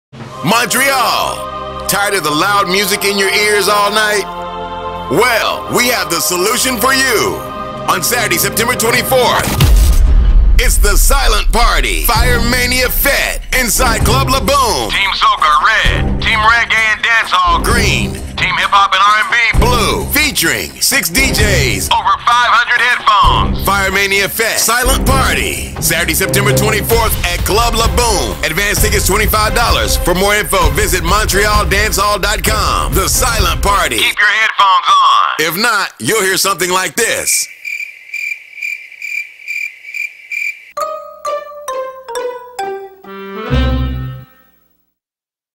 FIREMANIA FETE - SILENT PARTY
Montreal's first ever urban Hip-Hop, Soca, Reggae & Dancehall SILENT PARTY for the annual FIREMANIA FETE presented by Fire Empire Productions in association with Montrealdancehall.com.

Check it out Saturday September 24 inside La Boom featuring DJ Fire Jones, MAWGA K, DJ Kid, DJ Viza, Talent Squad & Color Squad